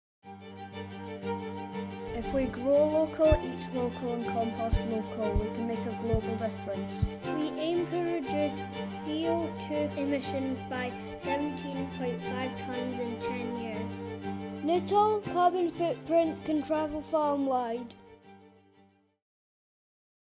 Raasay Primary - Earth Hour Challenge
In support of Earth Hour 2017 Raasay Primary submitted the following video as part of The Highland Council’s Earth Hour Challenge.